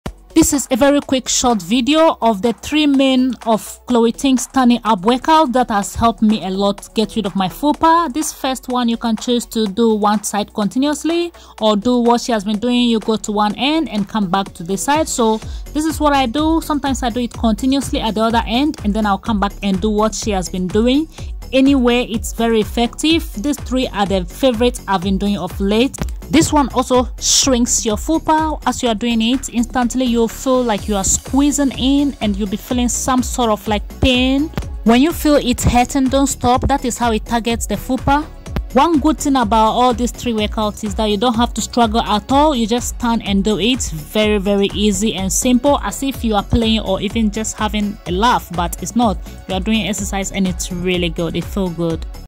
This 3 CHLOE TING STANDING ABS WORKOUT killed my Fupa
This 3 Chloe Ting Standing abs workout  killed my fupa, Just a quick one to let you know that yes the Chloe Ting standing abs workout are effective because it has helped me alot.

Ethnicity:   Ghanaian